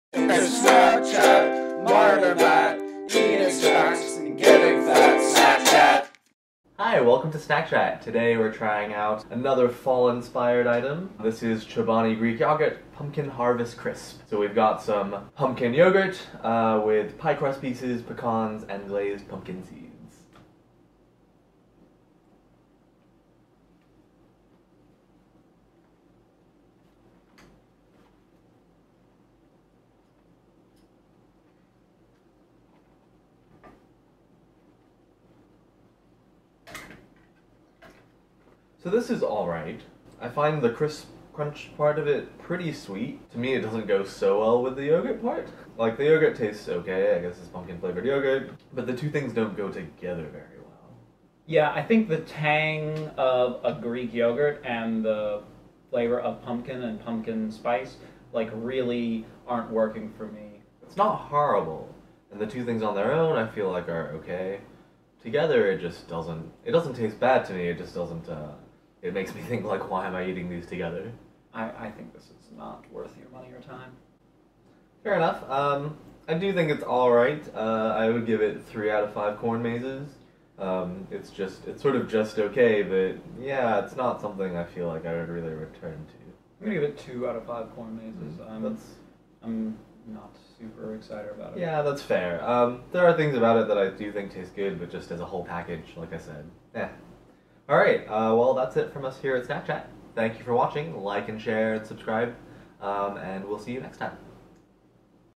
Chobani Pumpkin Harvest Crisp
Pumpkin spice yogurt? Might as well try that while we run down all the pumpkin flavored things out for the fall.

Matt gives it 2/5
Myron gives it 3/5